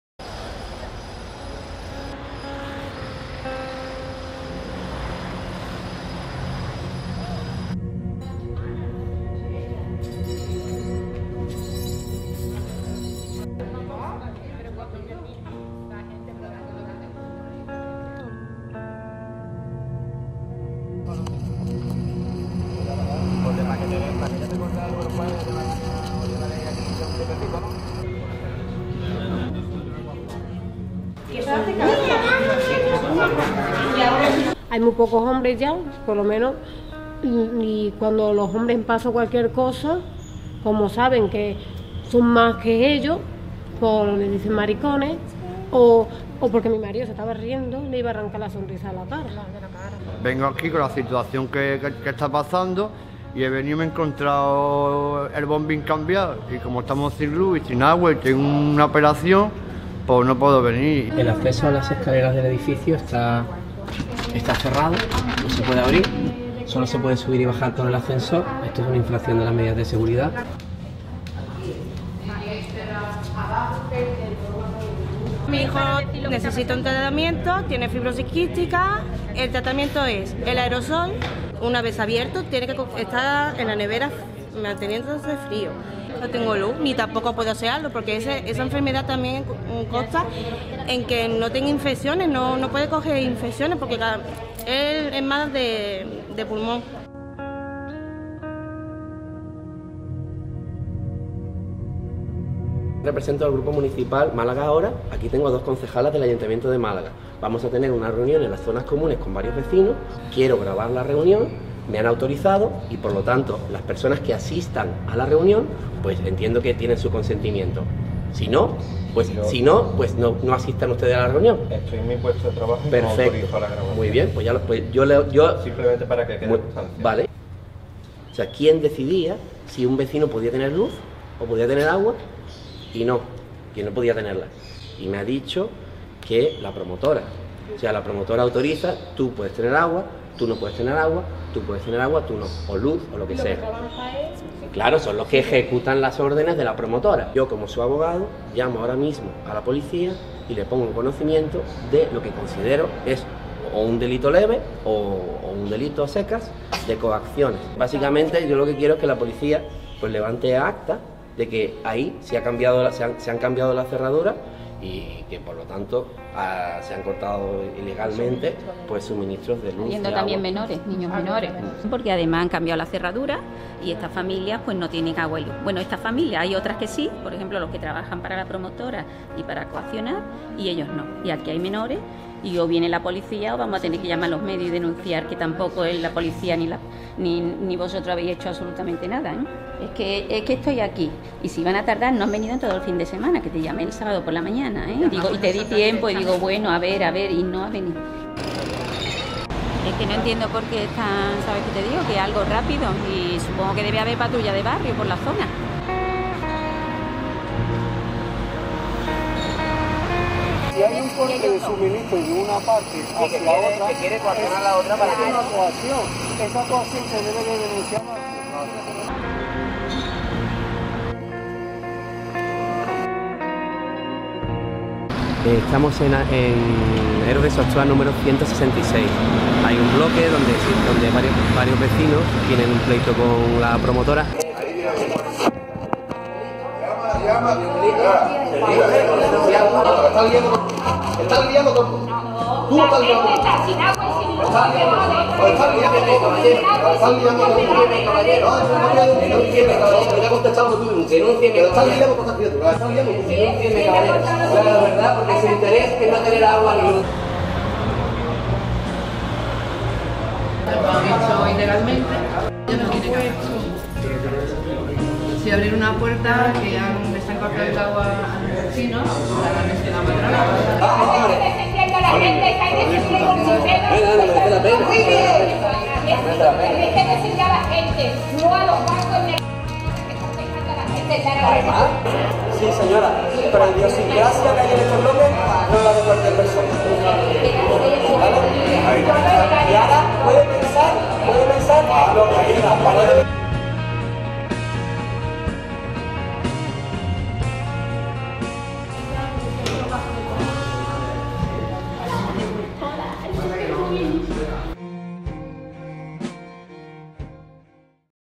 Familias coaccionadas sin luz y ni agua
Denunciamos las coacciones a las familias sin hogar que ocuparon el bloque de C/ Heroes de sostoa, 166 (Málaga)